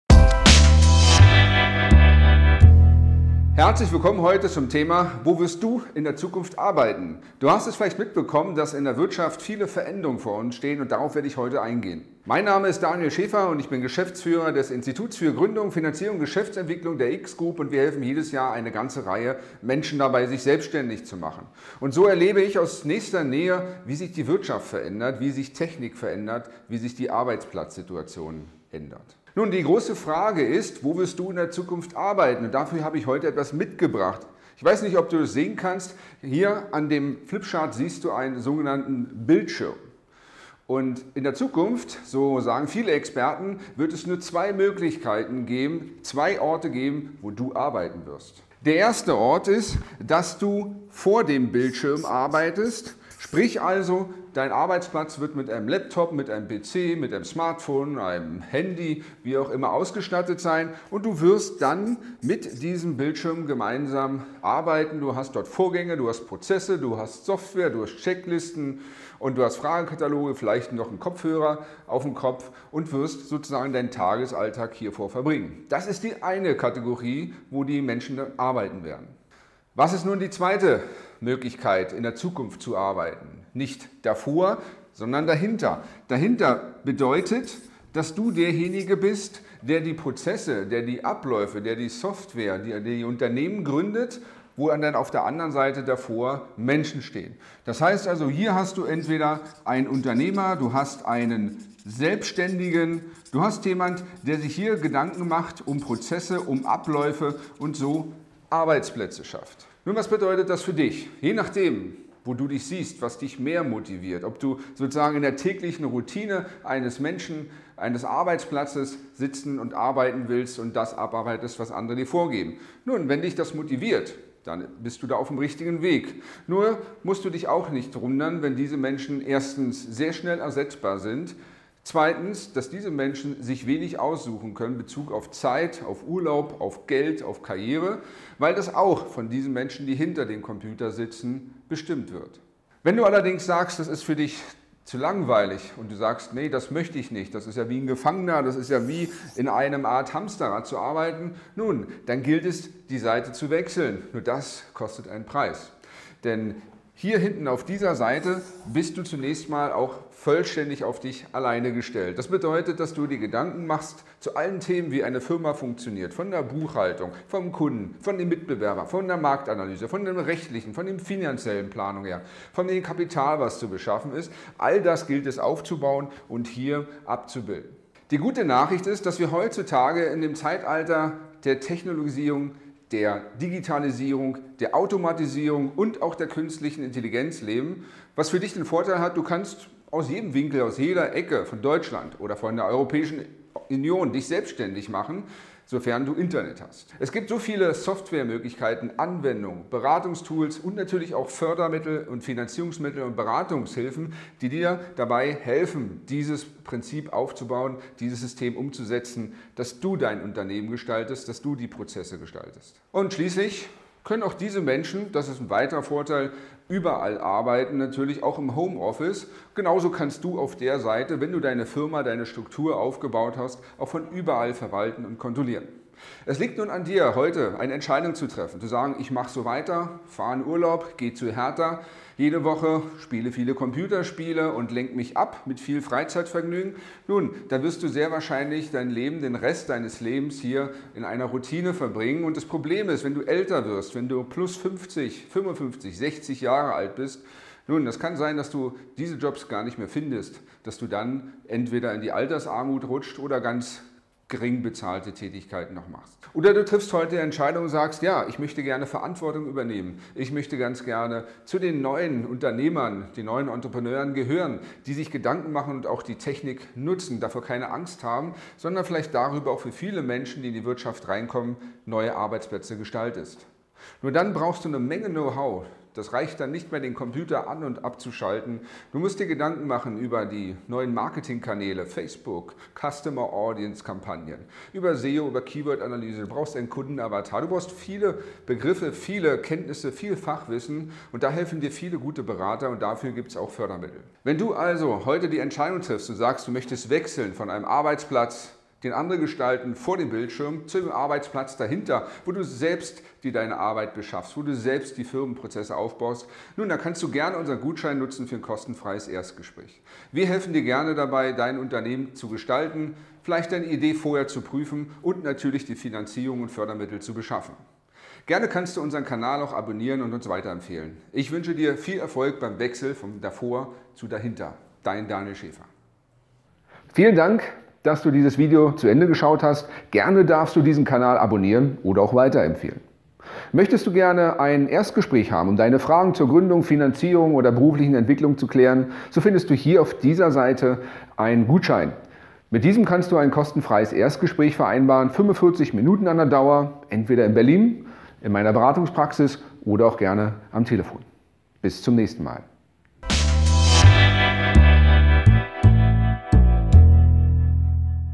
Wo wirst du in der Zukunft arbeiten?
Die Technik bringt viele Veränderungen mit sich. Auch die Wirtschaft ist davon nicht ausgeschlossen. Viele Experten sagen, dass es in der Zukunft zwei Orte geben wird, an denen du arbeiten kannst: VOR und HINTER dem Bildschirm. Was bedeutet das nun für dich? In diesem Video erfährst du es.